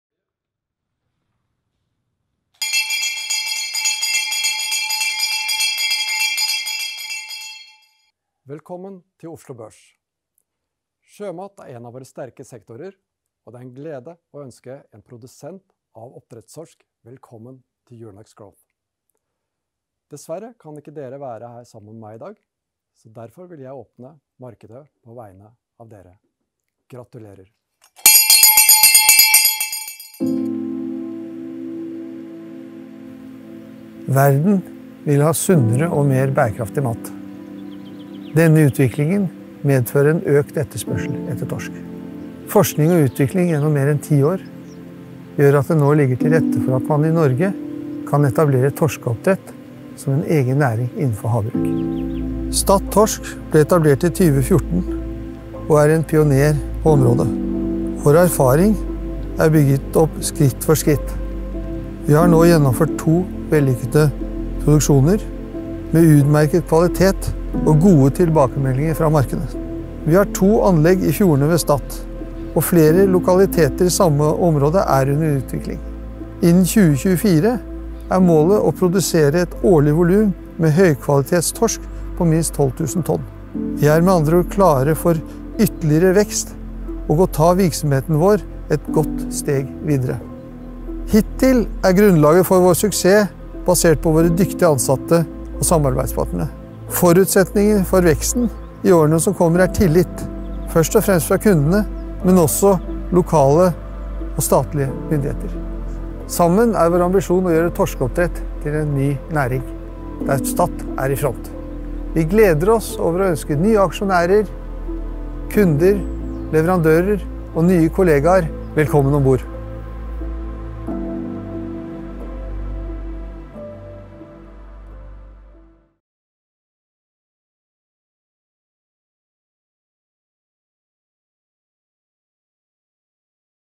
Statt Torsk - Euronext Growth
Statt Torsk listed on Euronext Growth Oslo 23 April 2021 | Statt Torsk notert på Euronext Growth Oslo 23. april 2021